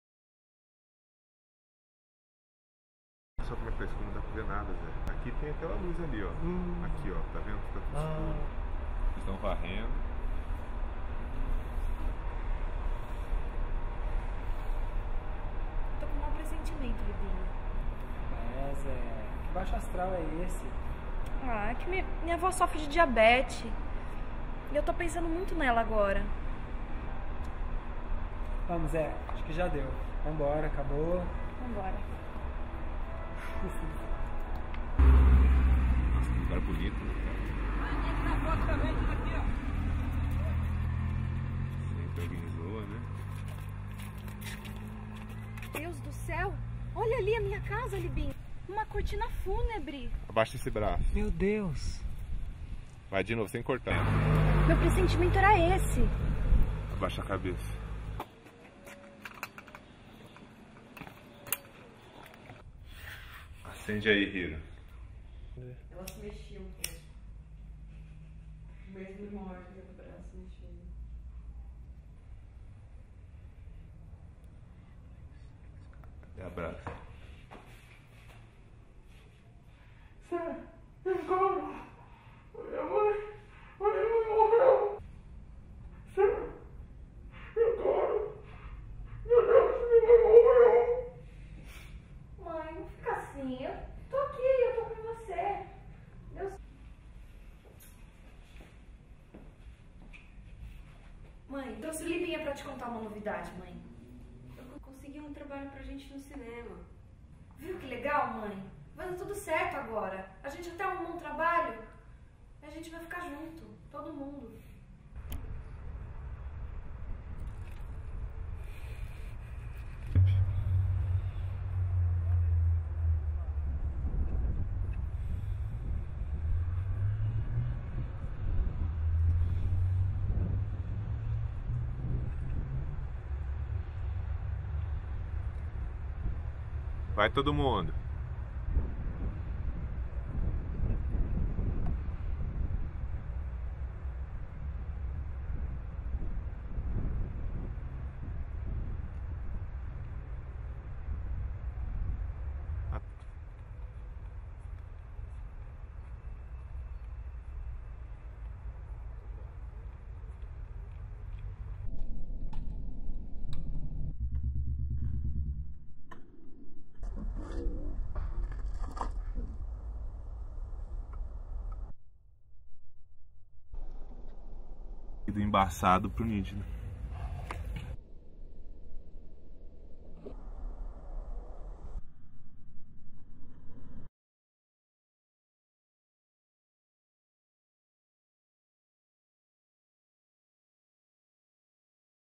Delírios de um cinemaníaco - Infância - Teste de direção - novas cenas
Teste de direção da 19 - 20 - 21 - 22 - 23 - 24 - SEQ da Infância do roteiro Delírios de um cinemaníaco. As imagens desse filme servem de exercício para ensaiar, treinar e aprimorar o olhar da direção dentro do set da infância que acontece nos dias 30 e 31 de Julho e 1, 2, 3 e 4 de Agosto de 2011. Para saber mais acesse